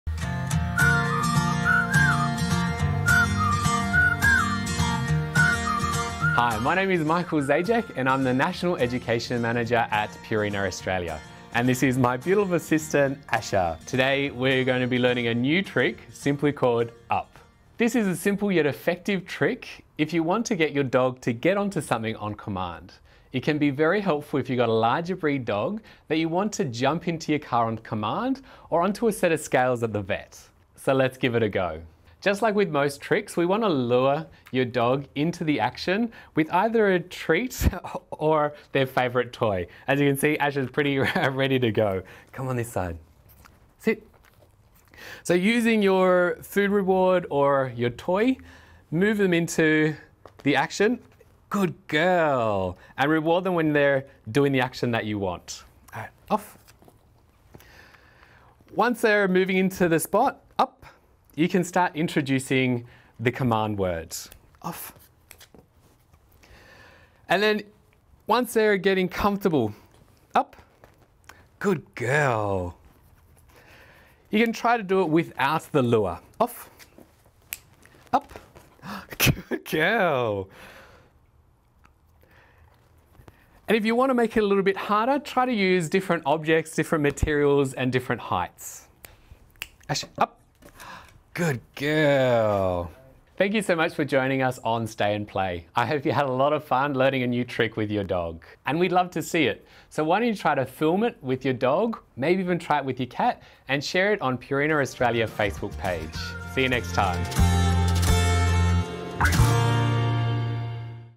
How To Teach Your Dog To Jump Up - Stay & Play Series
Purina's Stay & Play training series is designed for dog owners looking to teach their pet new tricks while we're at home.

The satisfaction you can feel from teaching your dog a new trick will help you bond with your pet, as well as stimulate your furry friend both physically and mentally. Michael Zajac, National Education Manager at Purina, shows us how to master the "up" or "jump up" command. This is a fun and practical trick to teach your dog, young or old. Use this command to get your dog to jump up into your car, or to move them onto a set of scales at the vet. Using food treats or your dog's favourite toy is the easiest way to teach your dog this fantastic trick. Discover how easy it is to teach your dog important and exciting commands with the Stay & Play video series from Purina.